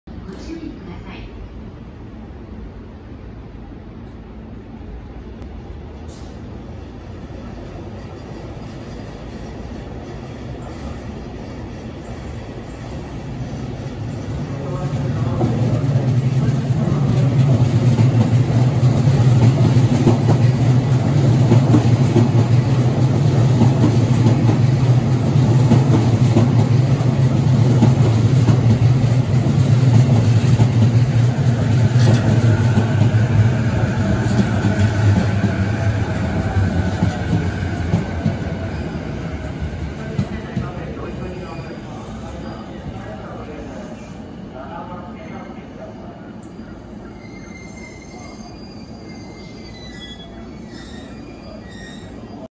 埼京線E233系大崎駅到着シーン
E233系埼京線大崎駅到着シーンです。
撮影が下手ですが、ご了承ください。